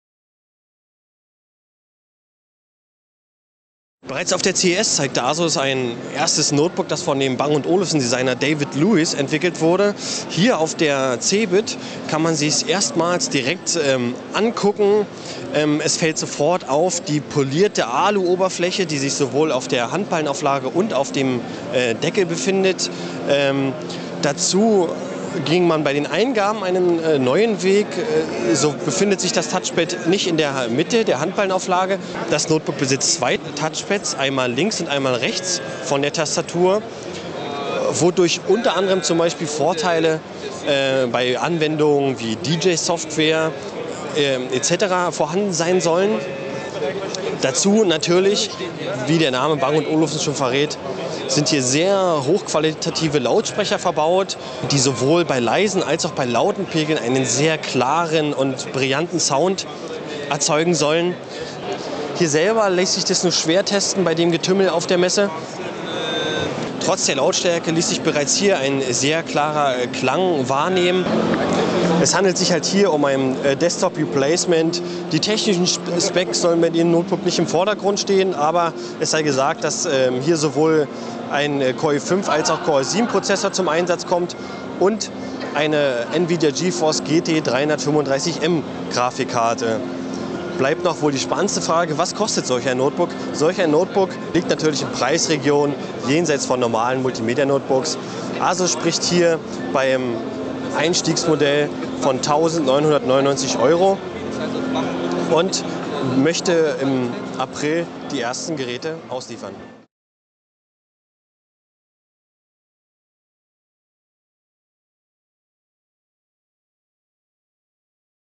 Asus NX90 auf der CeBIT 2010 gesichtet
CeBIT 2010: Mit dem NX90 läutet Asus die Zusammenarbeit mit dem Premium-Hersteller Bang & Olufsen ein. David Lewis, Designer bei Bang & Olufsen, hat bei der Entwicklung mitgewirkt.